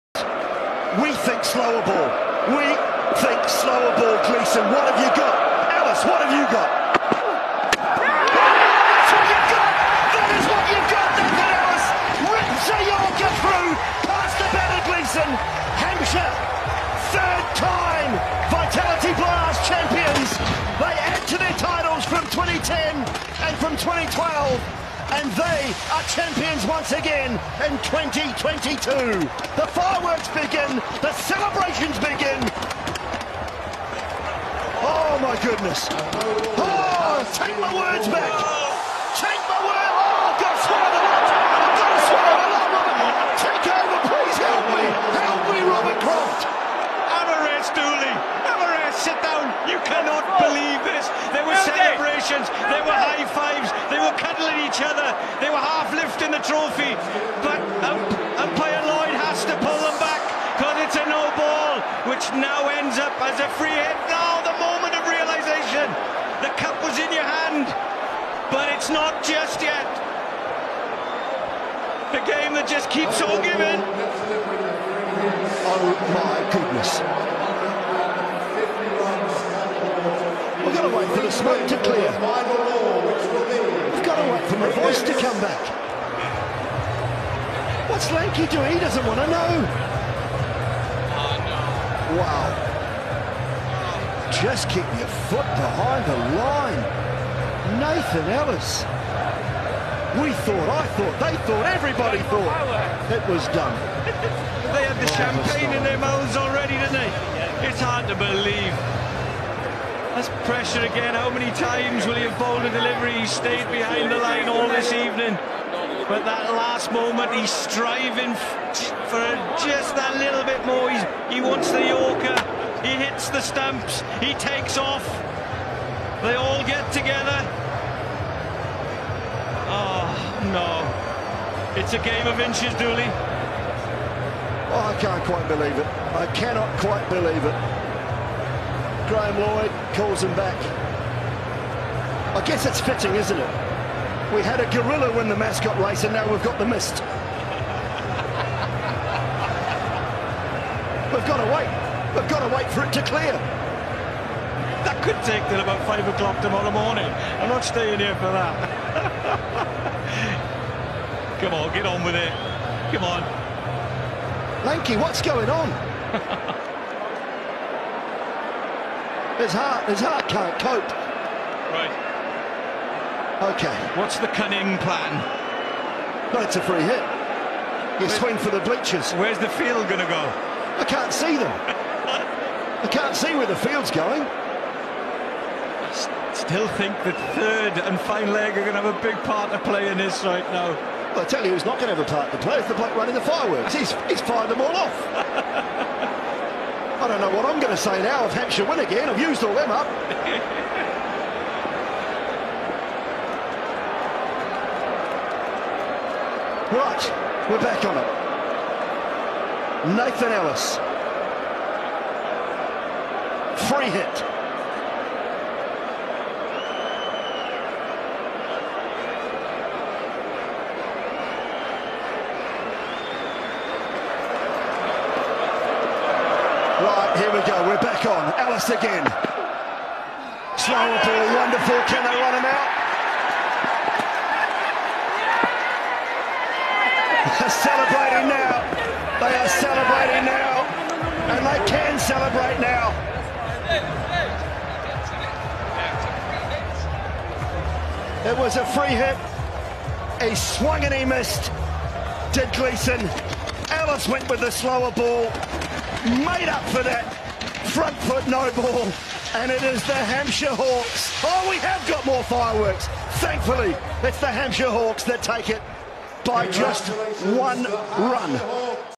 क्रिकेट मैच की सबसे महंगी नो बॉल पड़ी भारी,😱 लास्ट में जो हुआ वो देखो,🤣big bash league 2023 final
क्रिकेट मैच की सबसे महंगी नो बॉल,😱 लास्ट में जो हुआ वो देखो,🤣big bash league 2023 final match